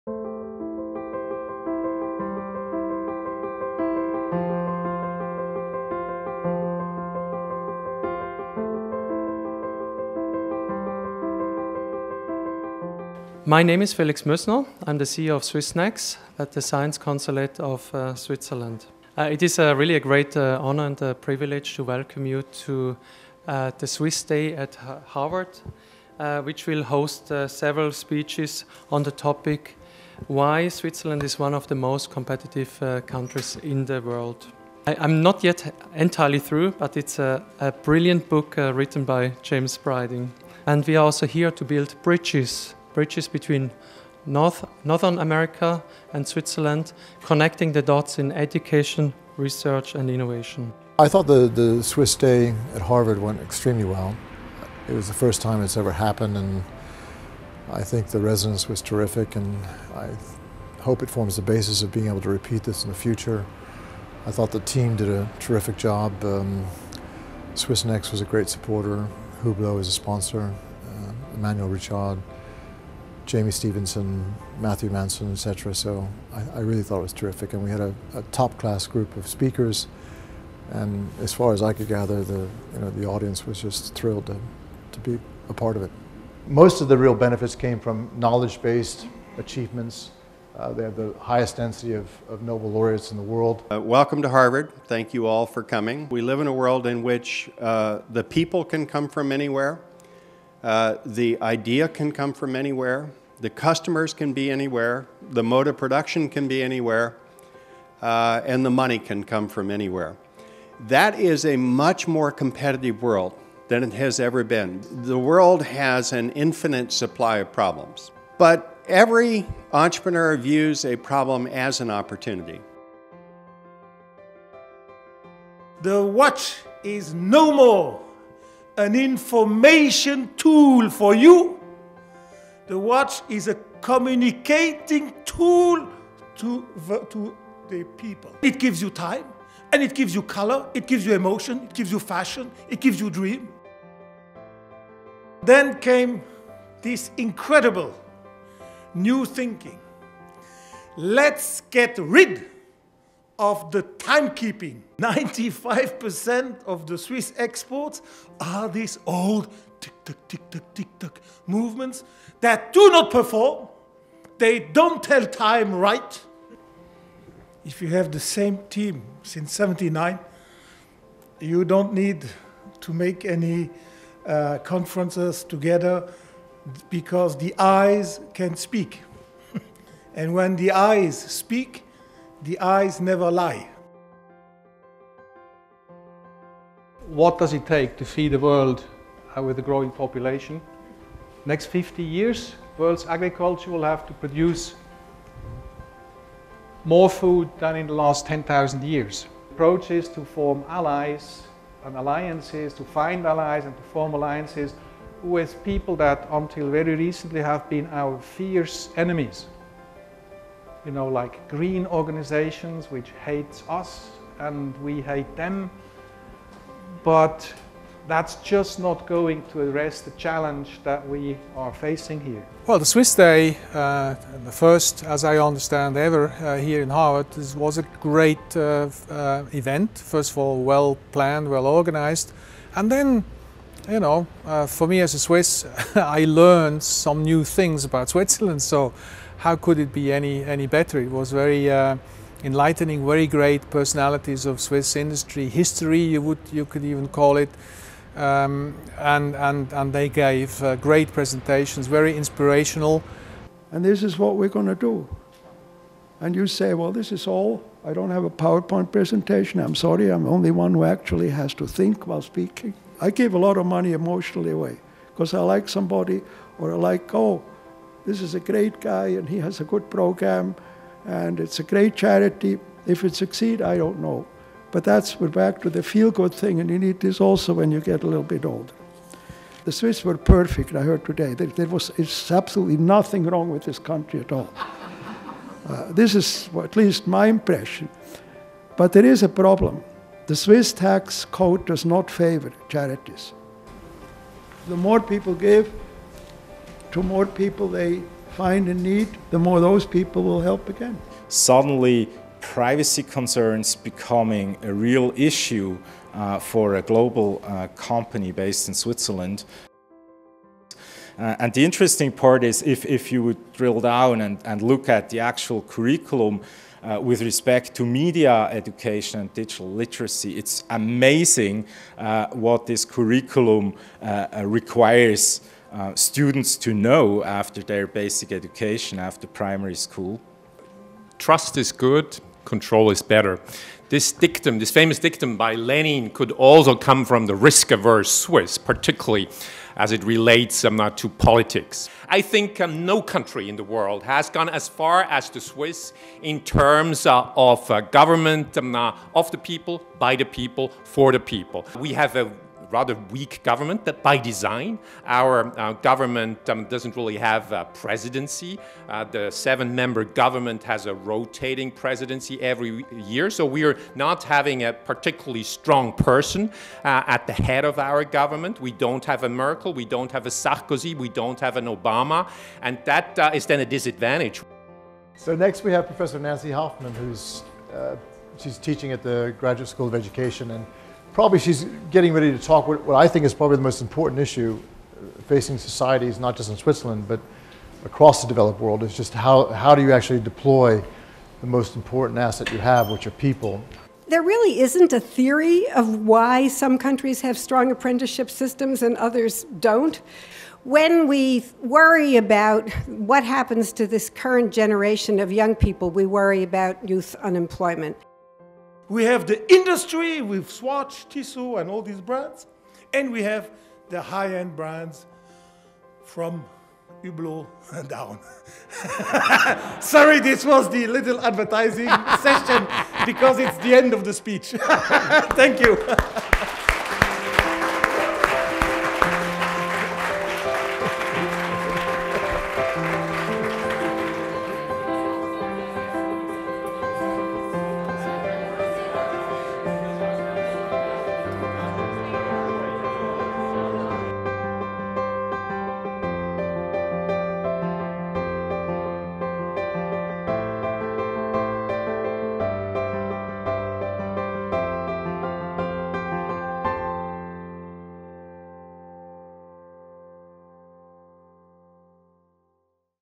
Swiss Day @ Harvard: Dancing Between the Elephants' Toes
"Why Switzerland is one of the most competitive countries in the world"
The unique Swiss Day at Harvard was held on 3 December 2013. Speakers included amongst others :
- Dr. Felix Moesner, swissnex Boston | Consulate of Switzerland
- Professor William A. Sahlman
- Dr. Urs Gasser
- Dr. Karl Hofstetter, Member of the Board Schindler Group
- Professor Nancy Hoffman
- R. James Breiding, Author of Swiss Made
- Jean-Claude Biver, CEO - Hublot
- Rolf Furter, Head of Global Product Development - Syngenta
- Prof. Charles Weissmann, Co-Founder -- Biogen
- Hans-Joerg Wyss